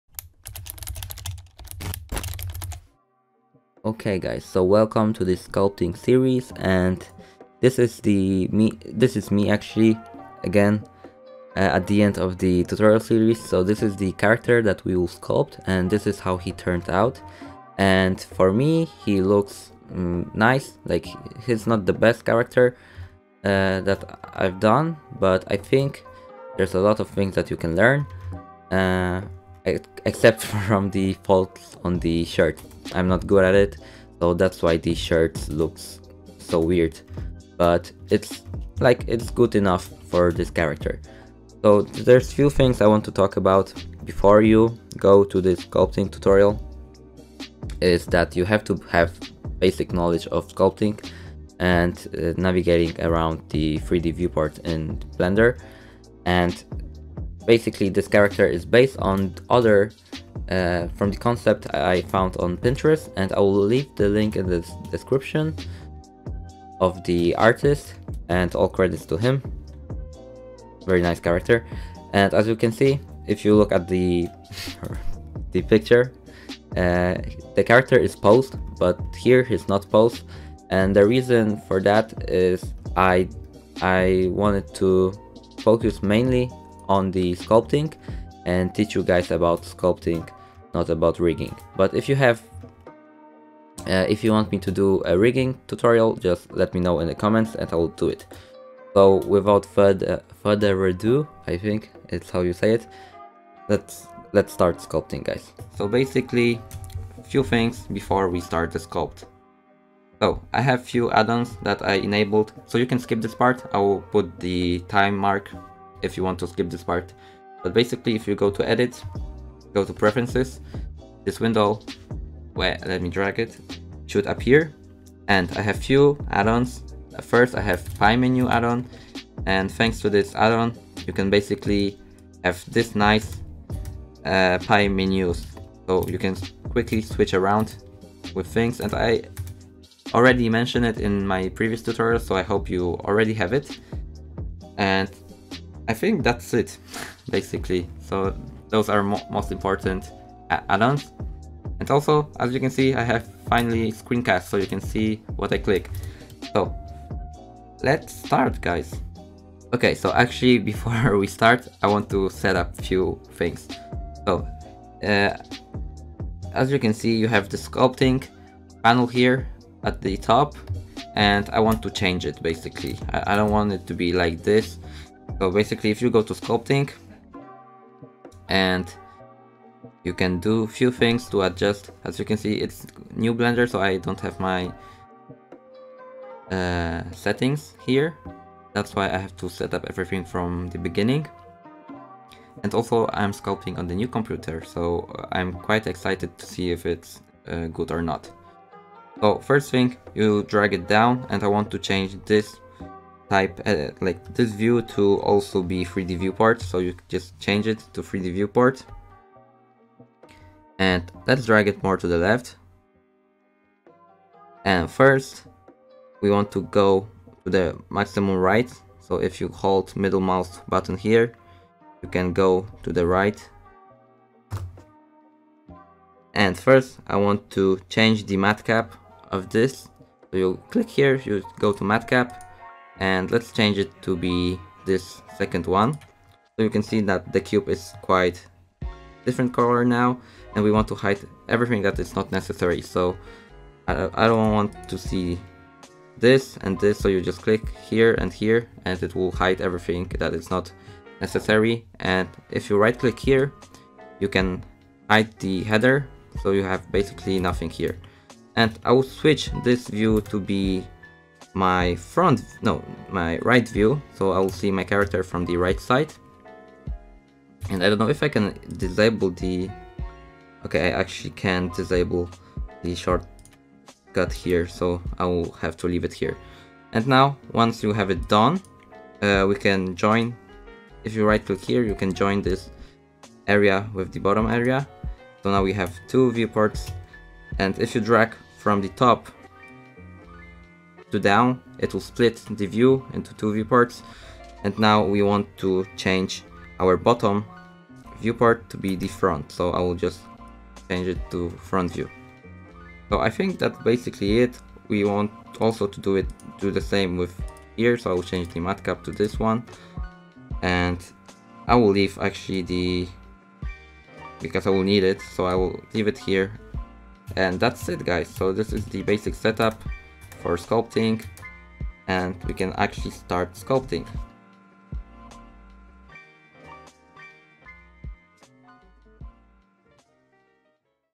How to sculpt Stylized Character Tutorial Series | Blender 2.81 | Introduction and Setup
Hello Guys, here is your tutorial series you all have benn waiting for i hope it will help you and you will like it. I spent quite a lot of time doing this for you my friends so i really hope you like it ;p

All credits to this guy, much love to you my friend

Anyway this is just intro video so next one is actually sculpting, so this one is quite boring :|